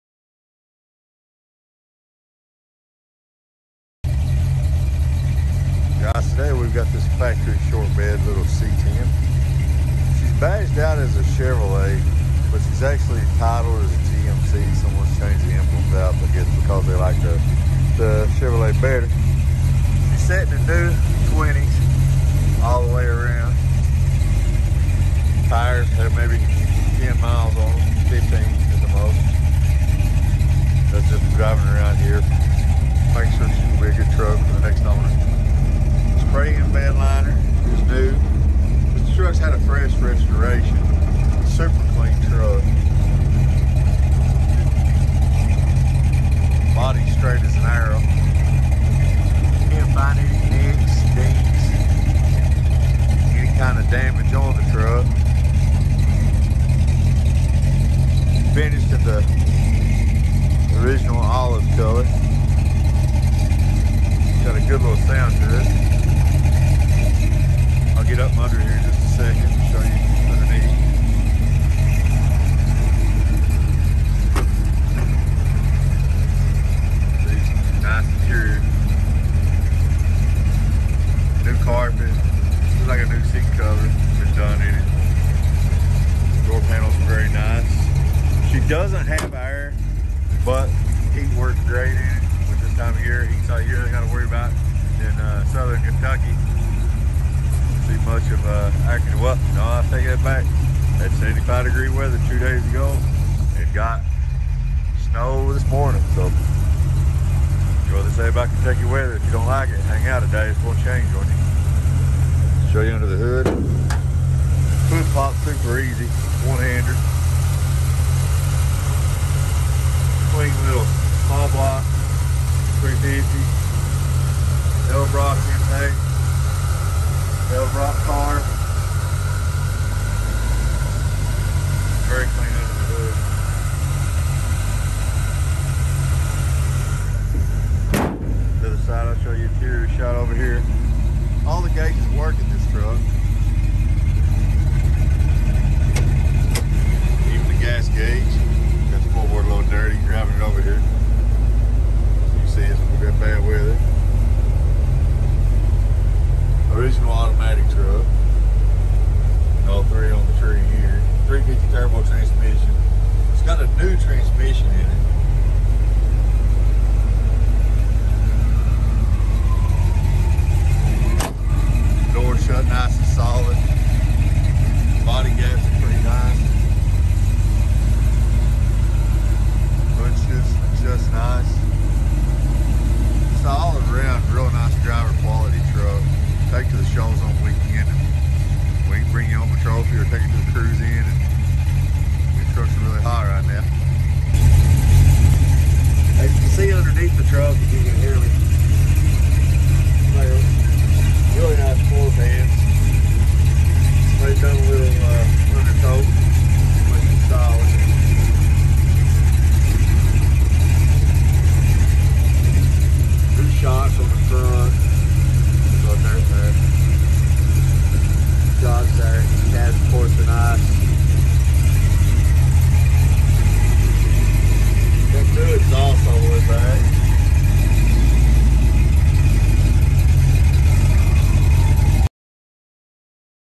Just Got In This Sweet 1971 C10!
1971 GMC that someone has changed out the front grille and emblems to Chevrolet. Strong running 350 sbc with newly remanufactured 700r4 transmission. Finished in a very nice Medium Olive with all new interior. She’s got new 20 inch wheels Along with new skins all the way around. New spray in liner to protect the bed from your  lawn chairs on the way to the cruise in.  Very clean under the hood with new shocks All the way around. Really nice subtle resto job. Priced at $34900